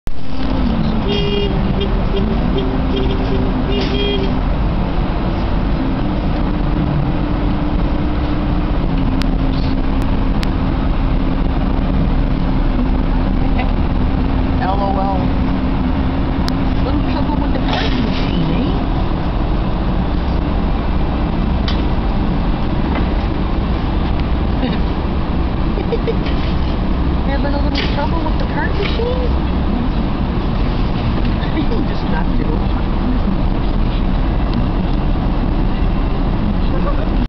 ELECTRIC CART MACHINE TROUBLE
Sat in my van just watching these young guys trying to push the machine in the snow..lol
I guess they didn't realize how funny they were..